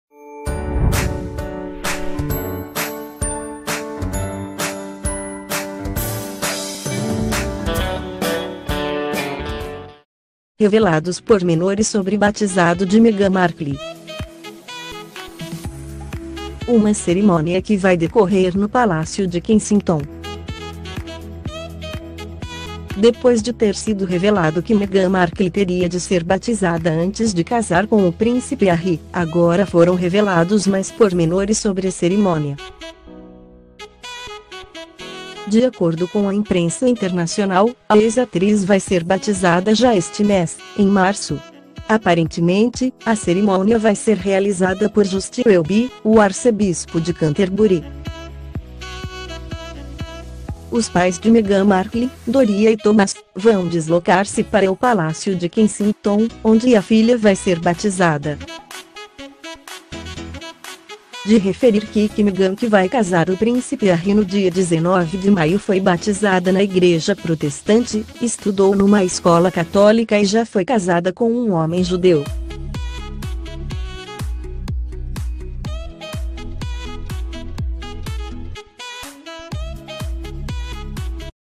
REVELADOS PORMENORES SOBRE BATIZADO DE MEGHAN MARKLE
Uma cerimónia que vai decorrer no Palácio de Kensington.